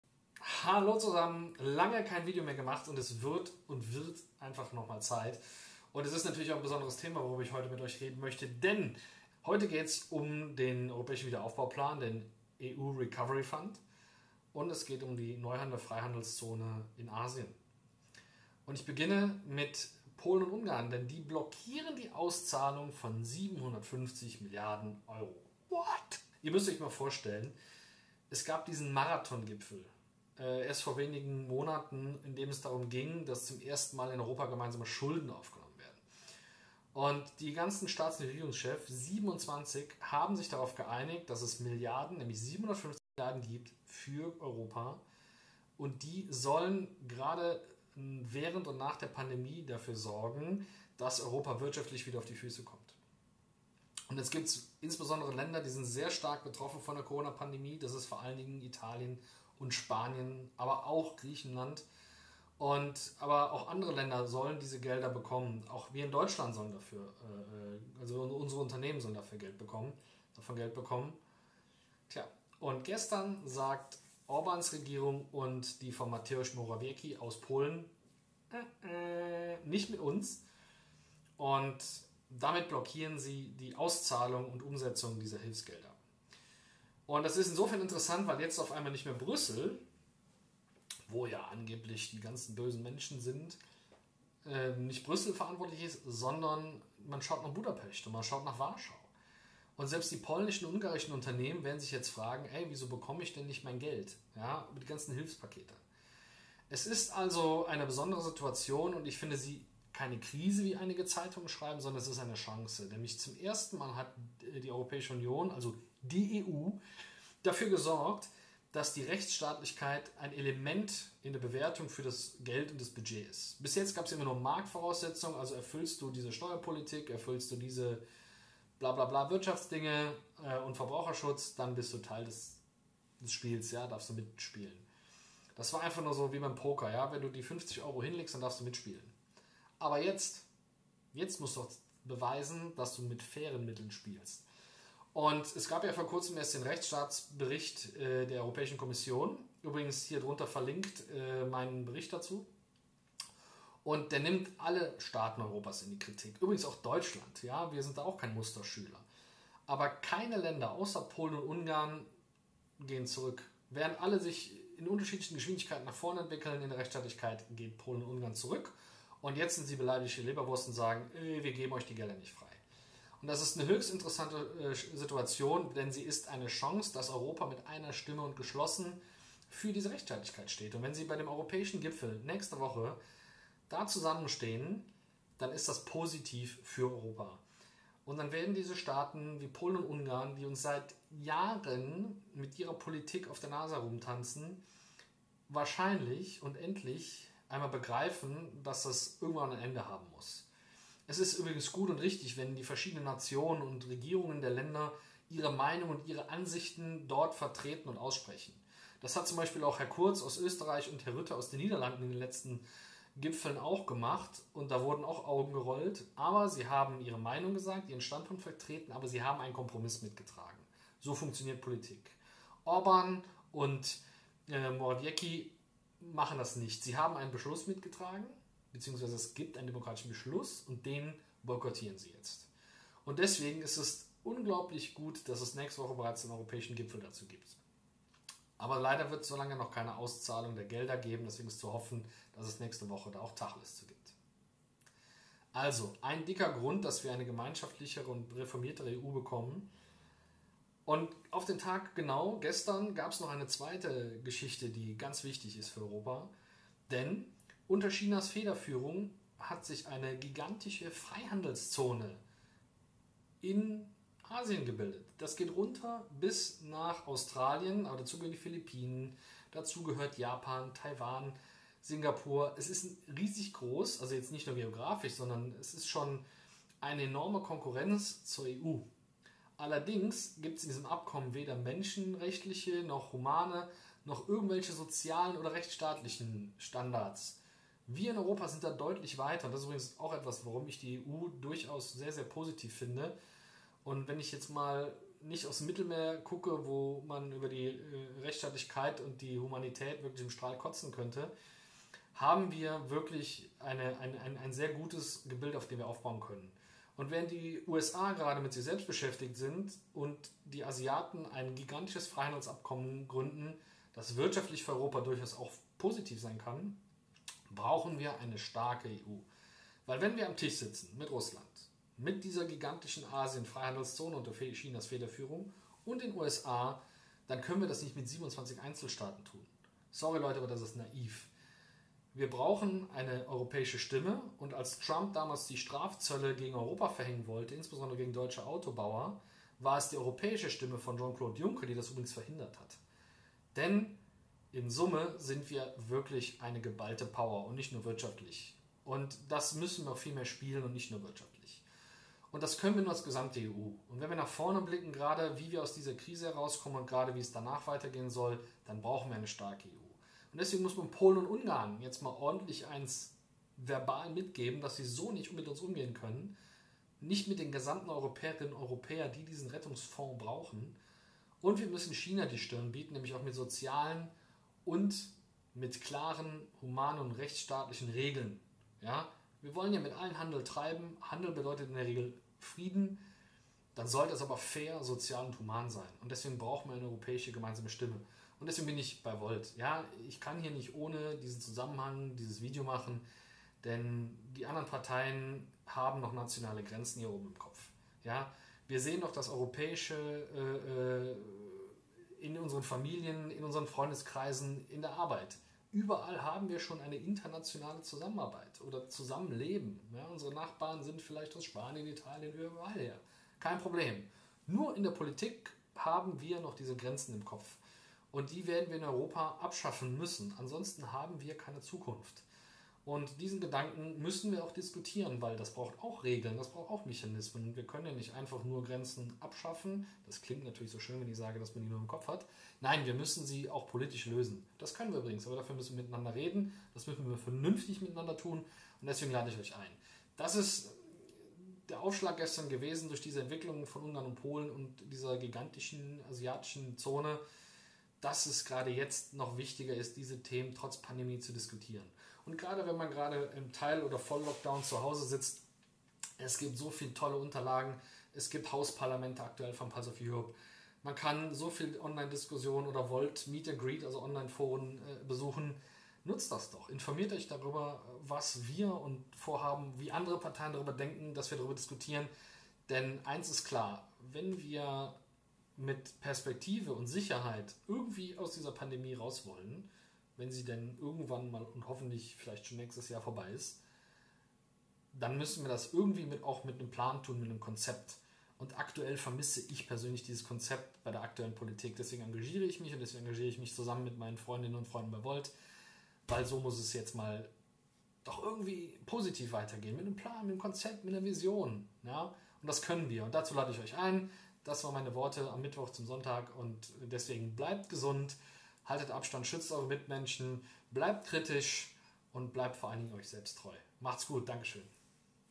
Europa ist die Antwort auf Polens, Ungarns & Chinas Politik
Die Regierungen von Ungarn und Polen verhindern die Auszahlung des EU recovery funds, des Europäischen Aufbauplans während und nach der Corona-Pandemie. Die 750 Milliarden Euro im EU Haushalt sind extra im Budget und nun verhindern Orban und Morawiecki die Auszahlung der Hilfsgelder.

Hier der Link zum im Video angesprochenen Bericht zur Rechtsstaatlichkeit der EU